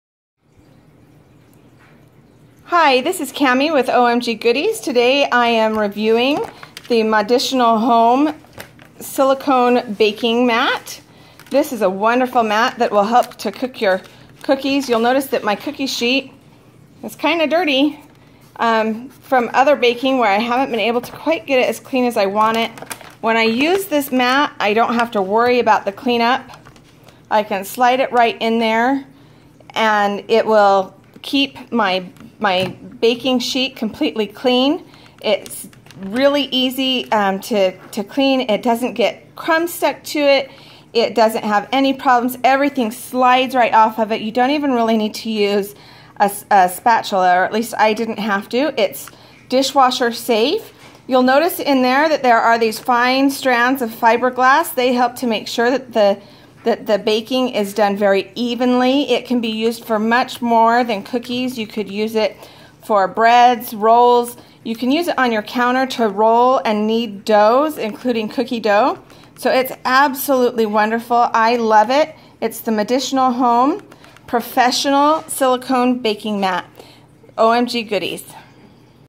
Moditional Home Silicone Baking Mat
See the Moditional Home Professional Silicone Baking Mat as reviewed by Kameo at OMG Goodies.